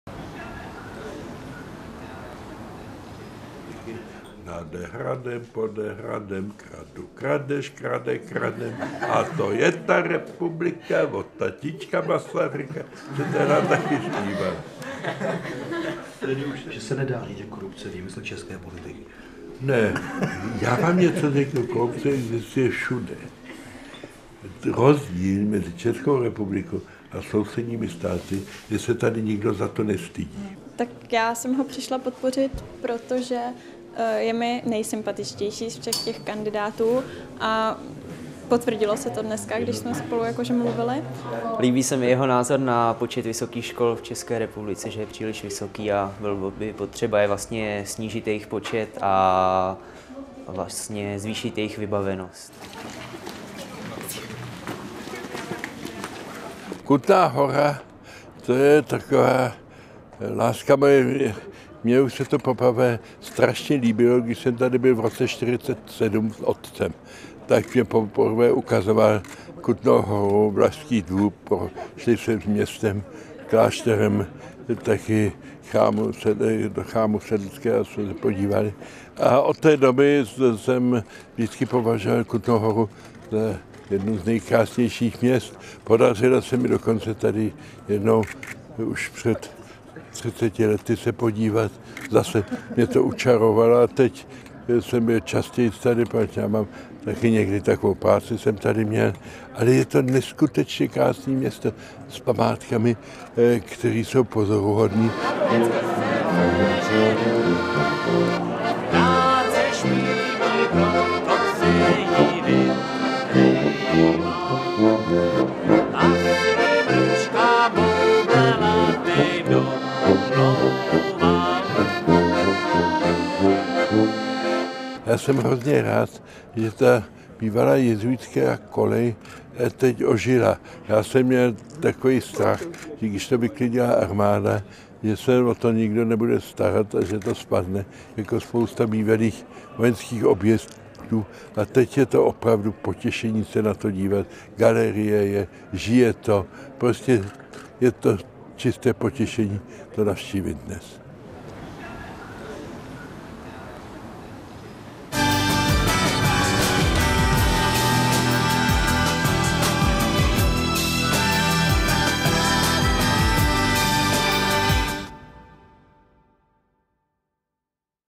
Karel Schwarzenberg: Kutná Hora je láska moje
Návštěva Karla Schwarzenberga v Kutné Hoře. Chcete-li se stát podporovatelem Karla Schwarzenberga, můžete se zaregistrovat zde